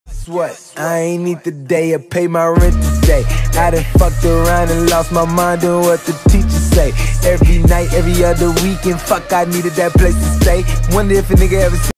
Firmino nice nutmeg skill and Klopp reaction
Liverpool Mancity 1-0